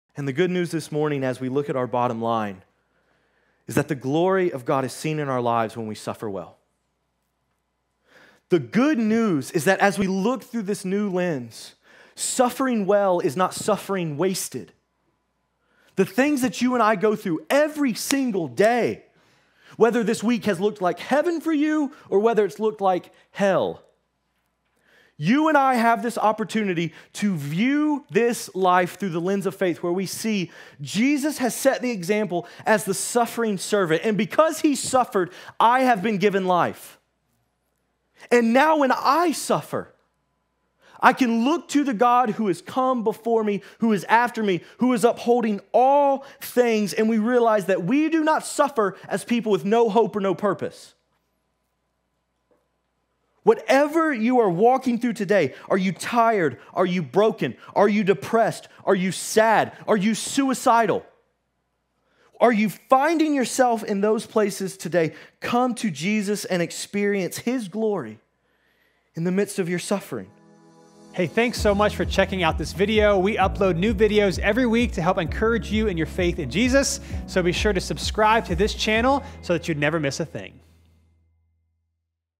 The glory of God is seen in our lives when we suffer well
Suffering gives us the opportunity to display God's glory in our lives.

To support this ministry and help us continue to help people meet Jesus and grow in a relationship with him